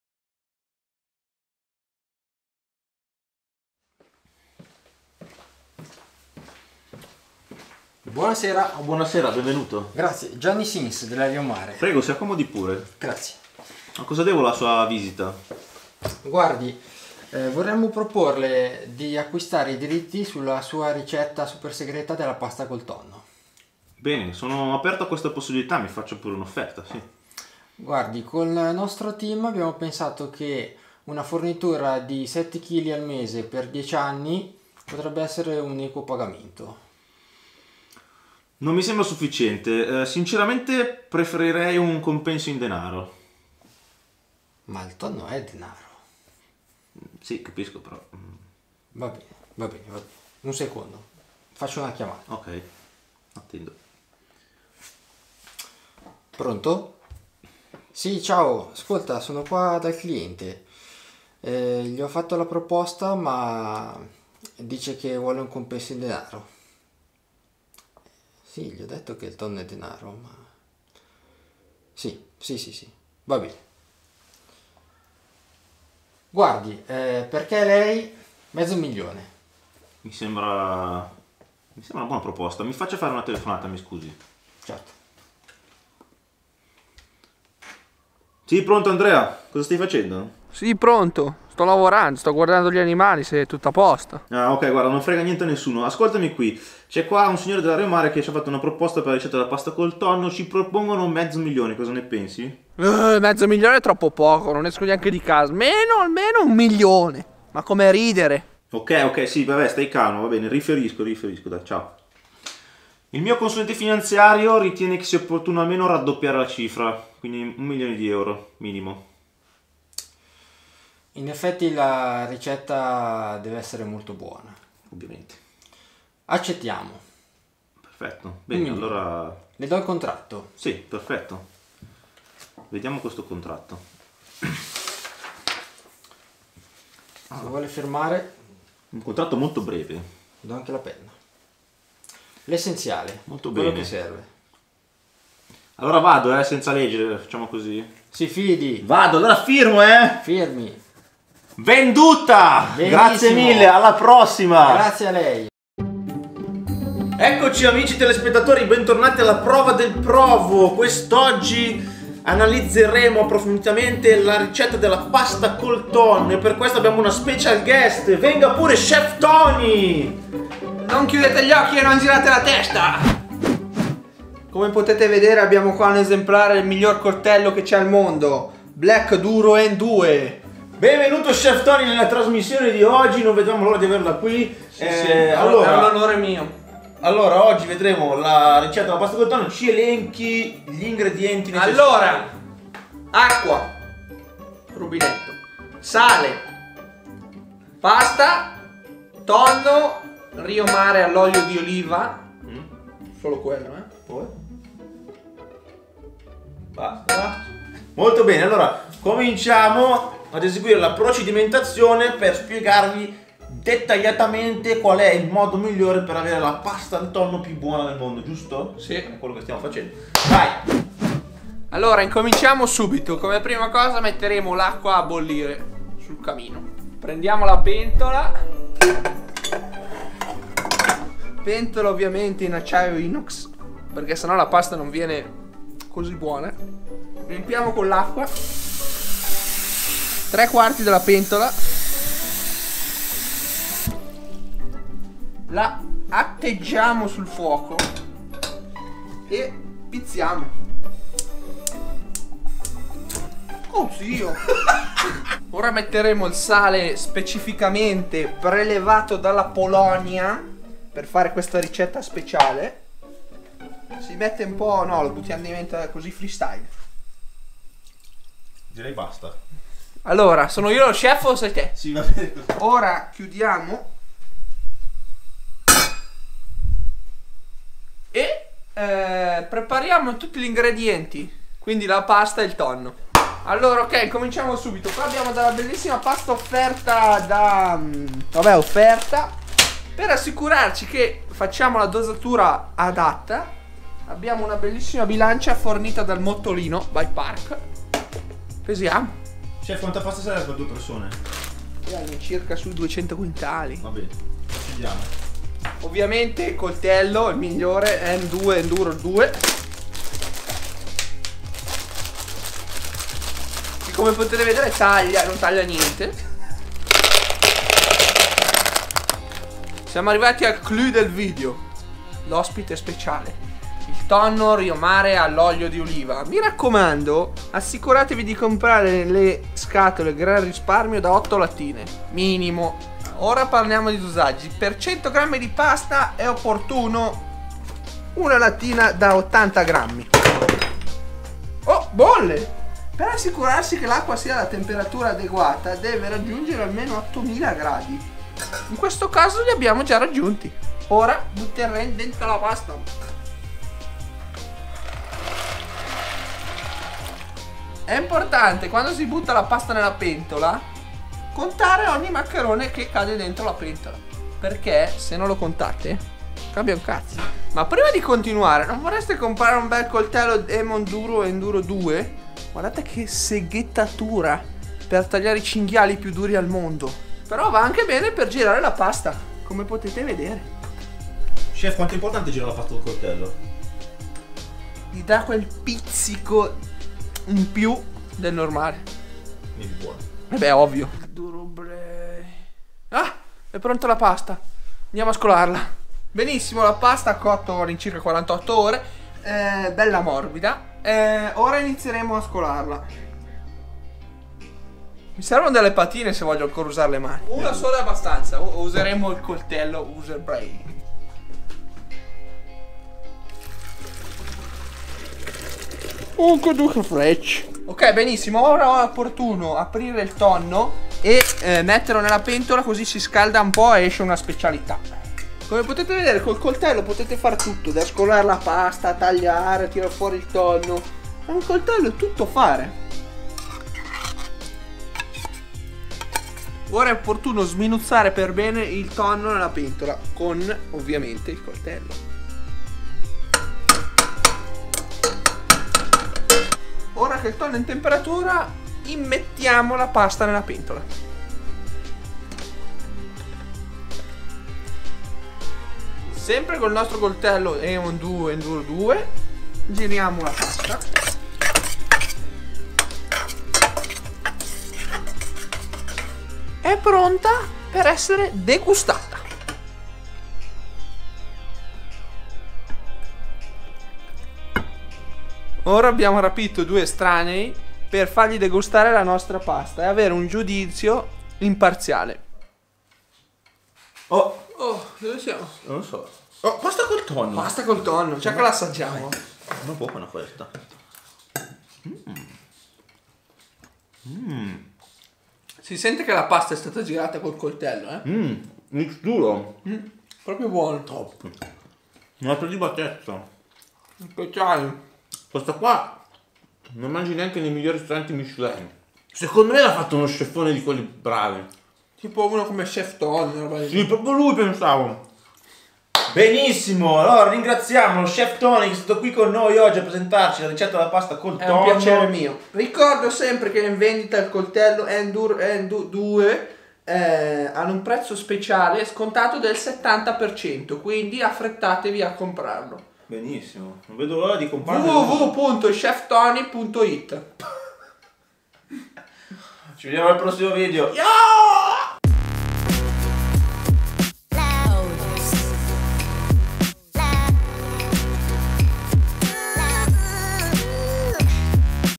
LA RICETTA SEGRETA DELLA PASTA COL TONNO! Feat Chef Tony!
A grande richiesta, dopo aver venduto i diritti della pasta col tonno originale, per la prima volta Chef Tony ci spiega passo passo come realizzarla al meglio alla Prova del Provo!ù
*Si ringrazia Simone dal Bosco per averci regalato il vestito da cuoco!!*